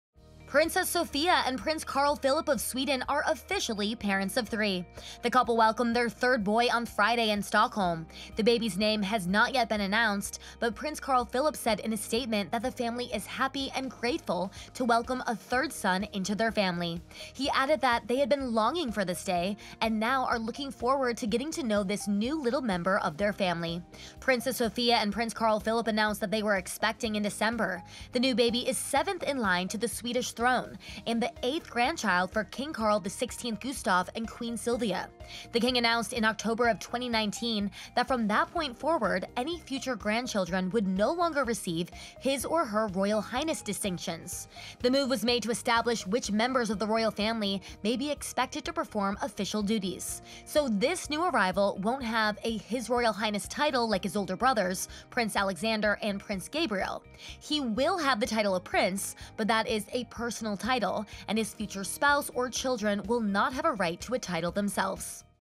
Royal Baby Alert! Princess Sofia and Prince Carl Philip of Sweden Welcome Third Child | PEOPLE
Princess Sofia and Prince Carl Philip of Sweden are officially parents of three boys!

The son of King Carl XVI Gustaf and his wife welcomed their third child, a baby boy, on Friday at Danderyd Hospital in Stockholm. That makes their sons, 4-year-old Prince Alexander and 3-year-old Prince Gabriel, big brothers!